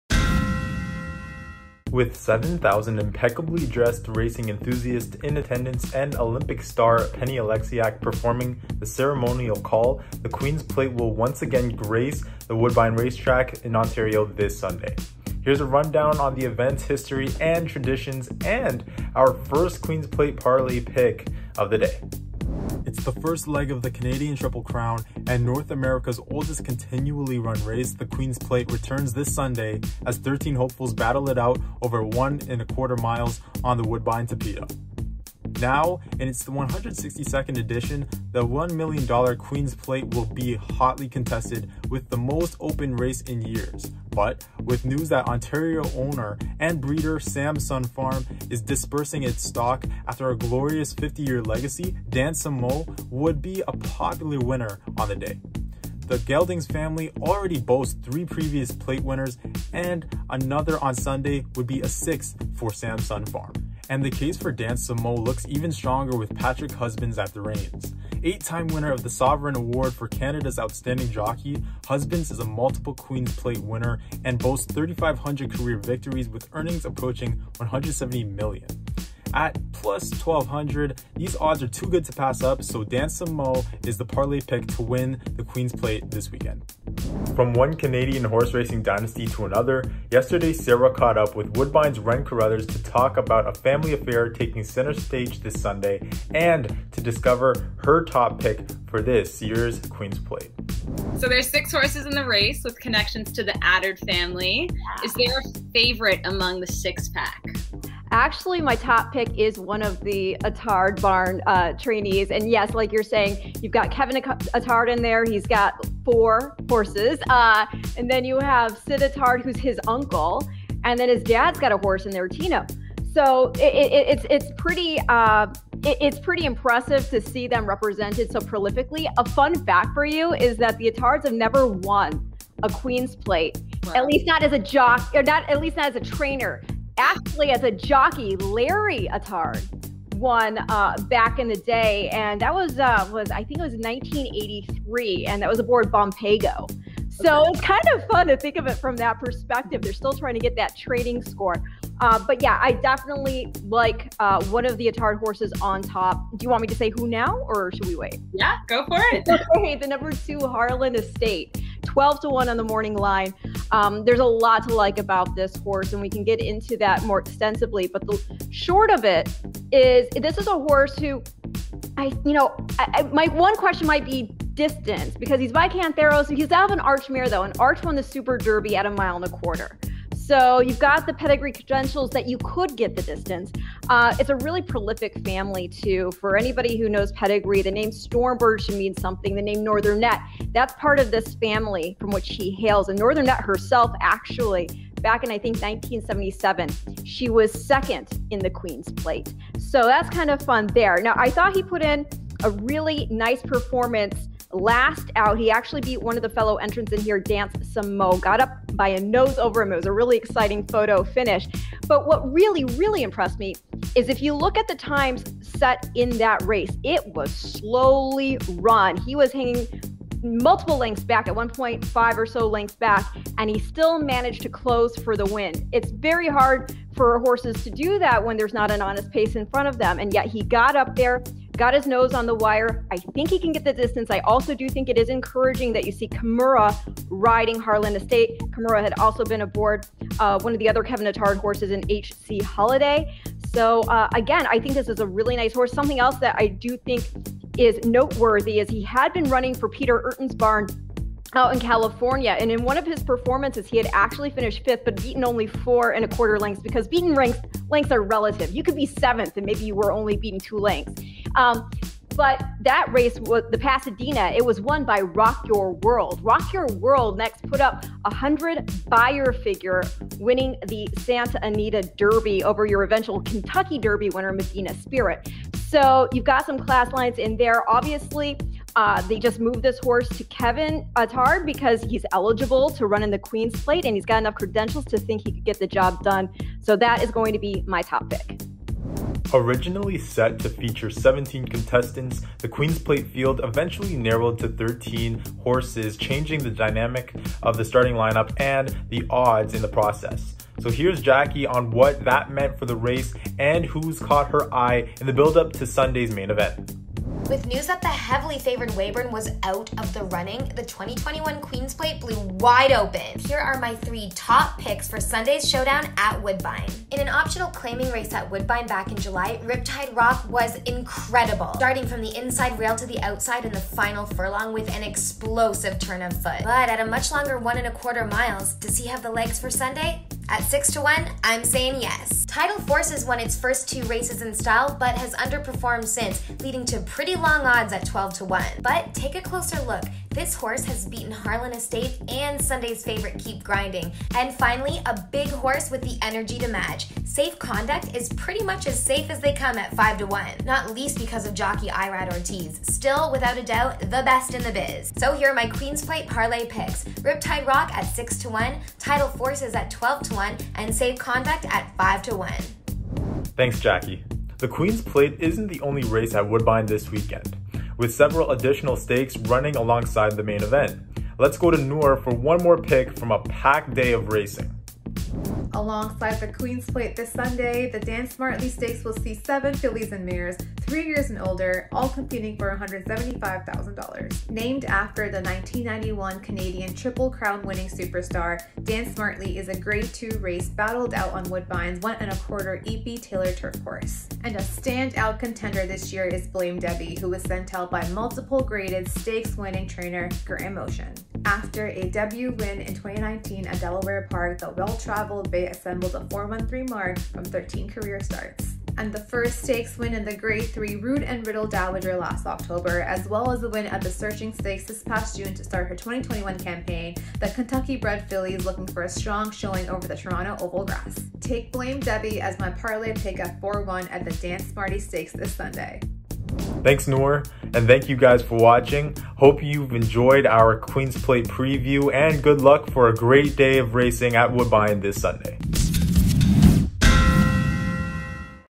Get Parleh-ready for the Queen’s Plate at Woodbine!
With seven thousand IMPECCABLY DRESSED racing enthusiasts in attendance, and Olympic star Penny Oleskiak performing the ceremonial call - the Queen’s Plate will once again grace the Woodbine racetrack in Ontario this Sunday! Picks, interviews and more...